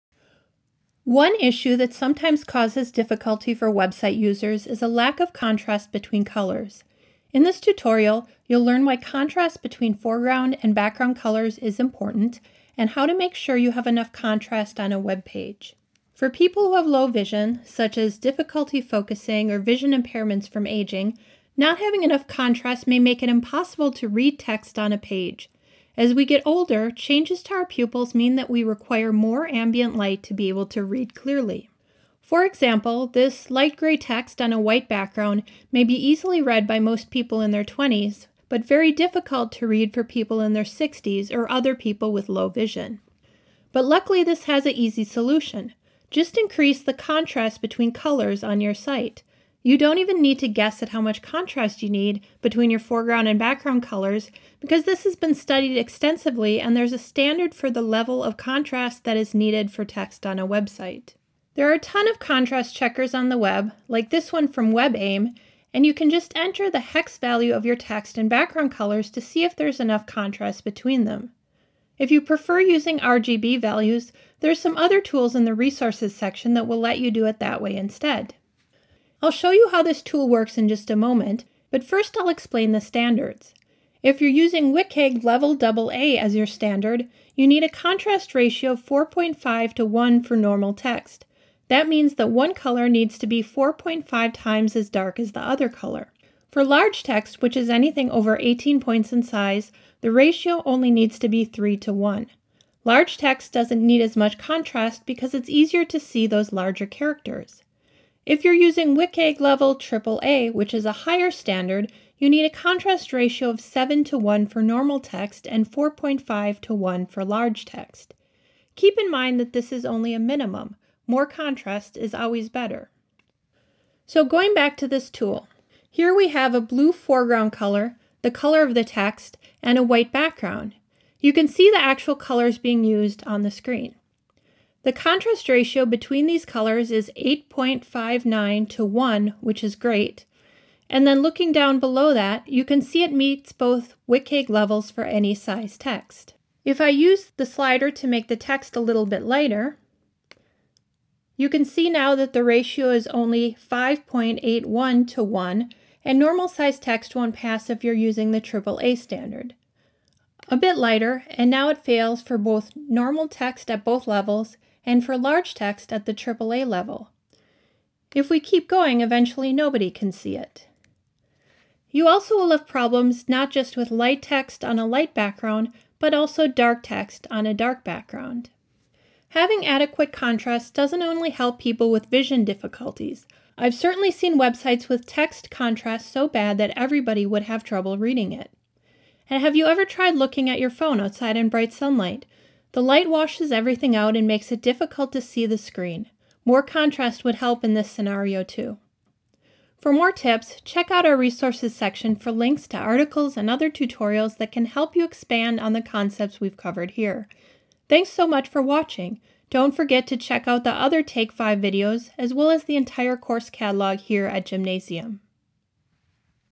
Improving Accessibility With Color Contrast
Discover how to identify whether the colors on your website have sufficient contrast between text color and its background in this tutorial by Clarissa Peterson, a Chicago-based designer who specializes in user experience and content strategy.

RESOURCES